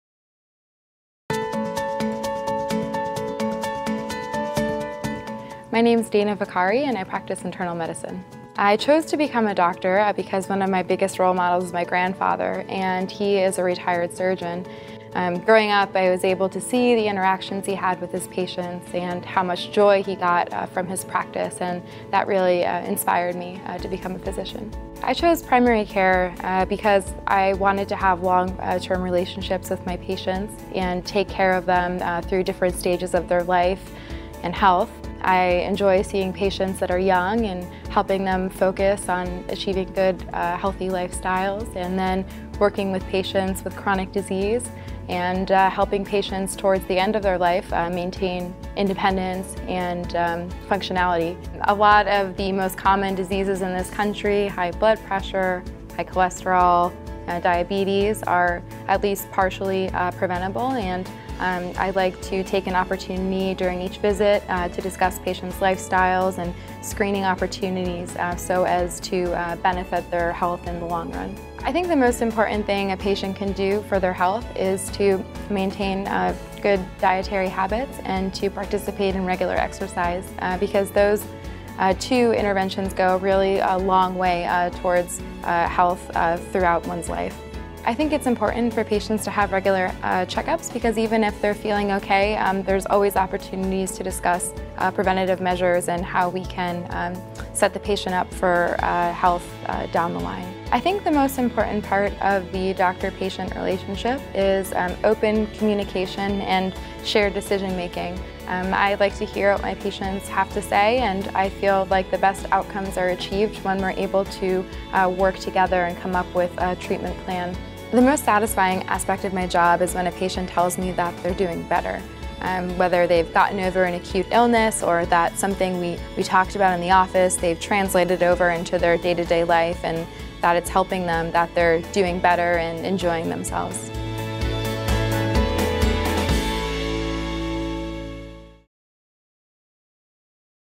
Dana Vaccari, MD, Internal Medicine
Dana Vaccari, MD, is an internal medicine doctor at Scripps Clinic, Torrey Pines. She emphasizes preventive care and a healthy lifestyle.